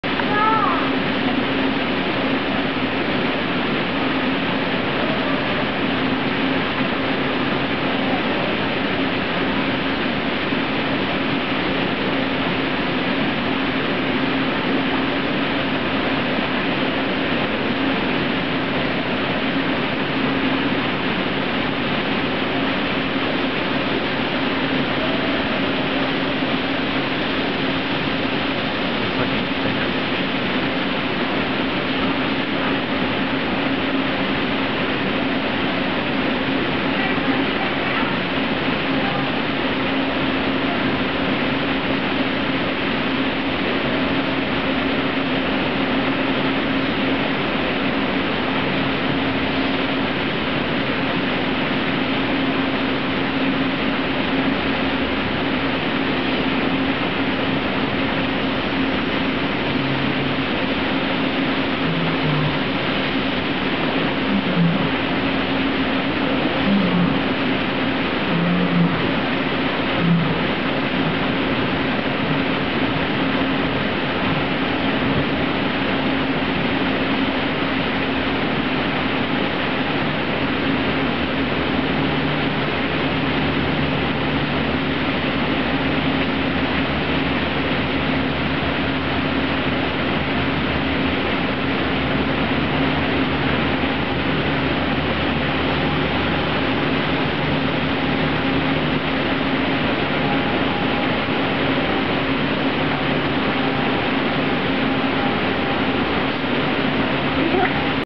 Philadelphia Zoo Ringtailed Lemurs
These ringtailed lemurs sit outside of the Primate House during the summer.

The like to watch visitors as much as the visitors like to watch them.